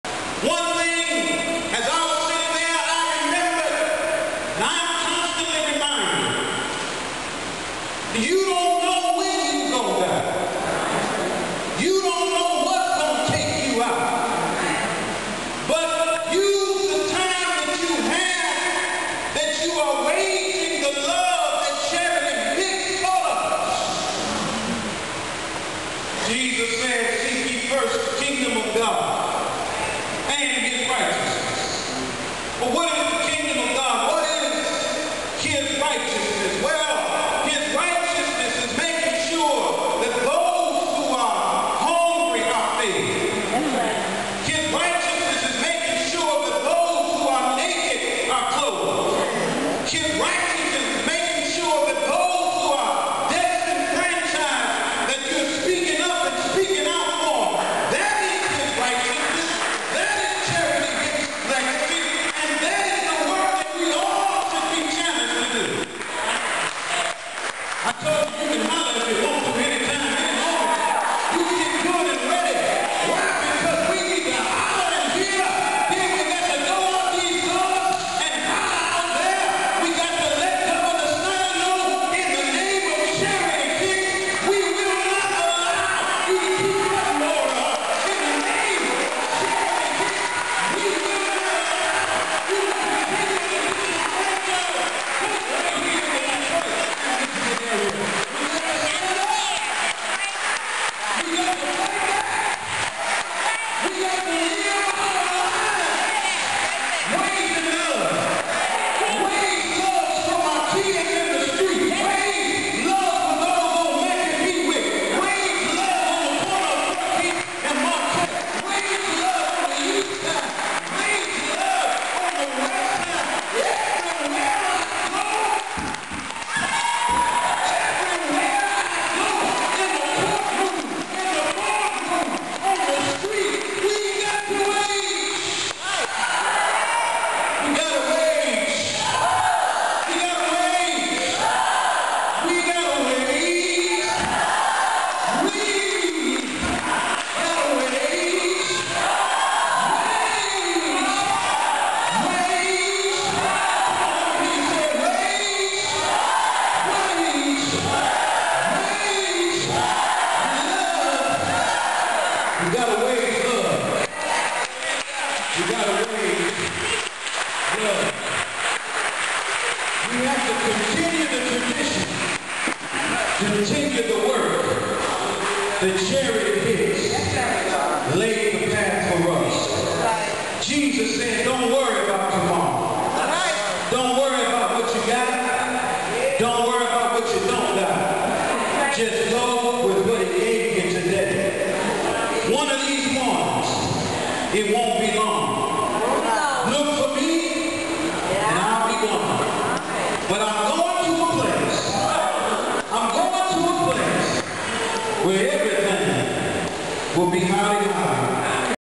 Wage Love, “In the name of Charity Hicks”, Eulogy snippet w/ Rev. Charles Williams II
Featured Speaker: Rev. Charles Williams II, Pastor of King Solomon Baptist Church; and President of Detroit Chapter of National Action Network (NAN).

This video clip was recorded on Saturday, July 19, 2014 during the “Homegoing Celebration of the Life of Charity Louise Darlene Hicks (June 30, 1969 – July 8, 2014)” at Church of the Messiah, 231 E. Grand Blvd., Detroit, MI.